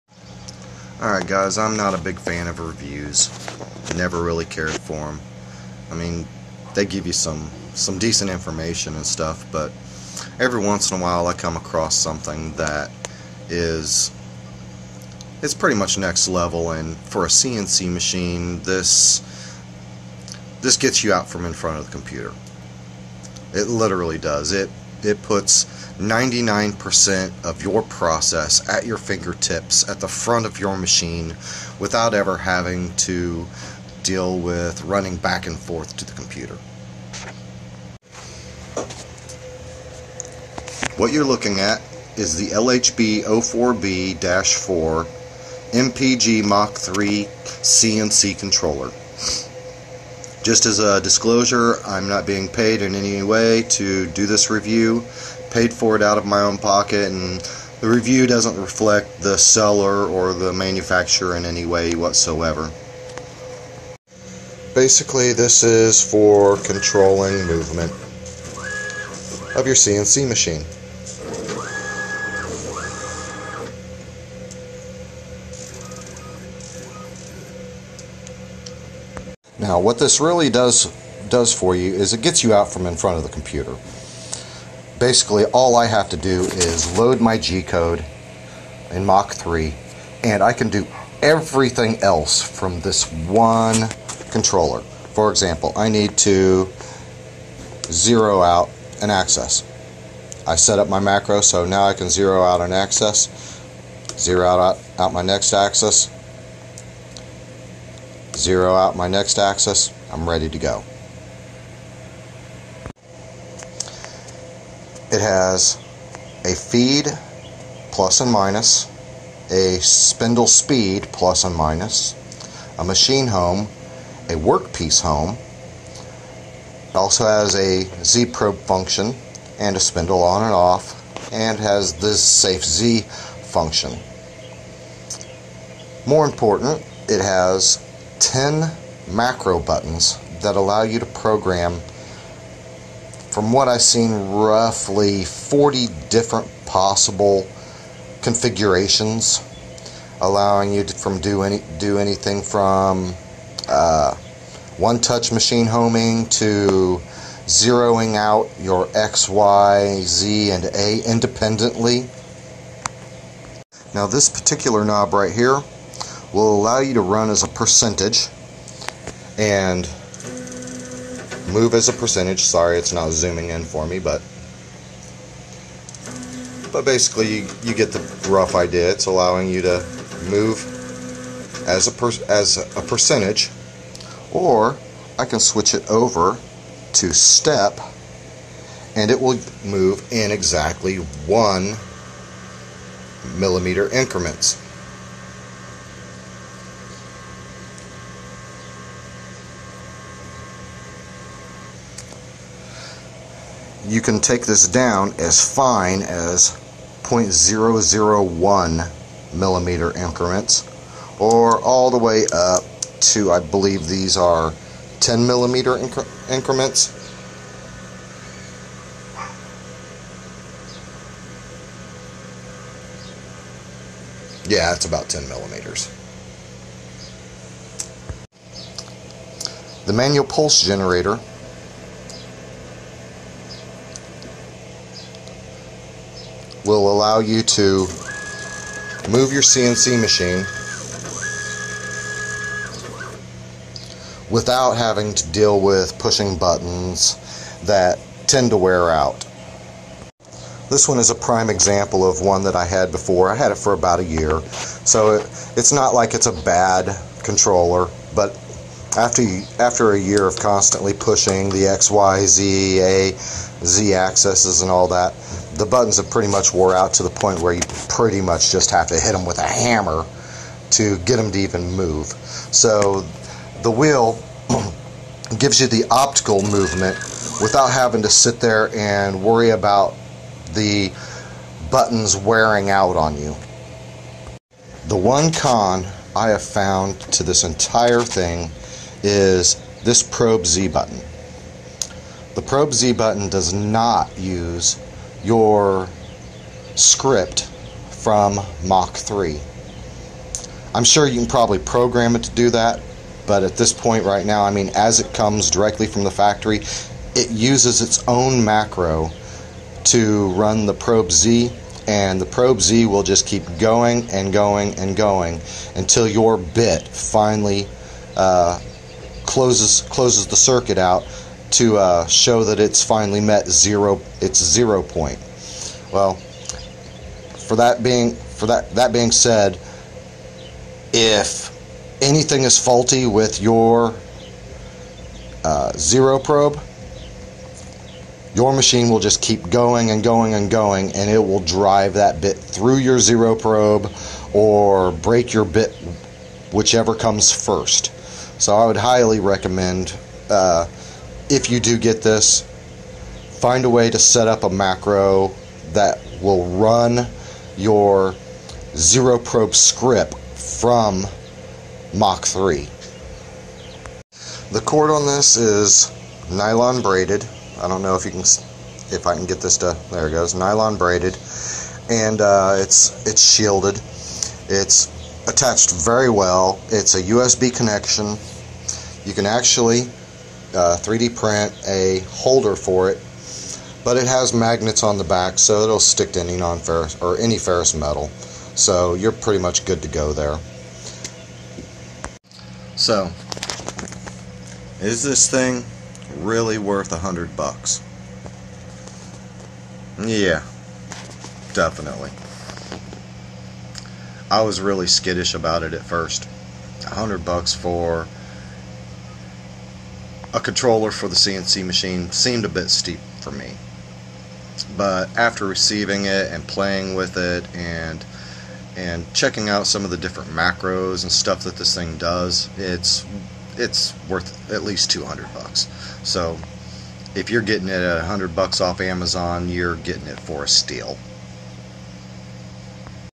Mach3 Mpg
Review of Lhb04b-4 Mach3 Mpg cnc controller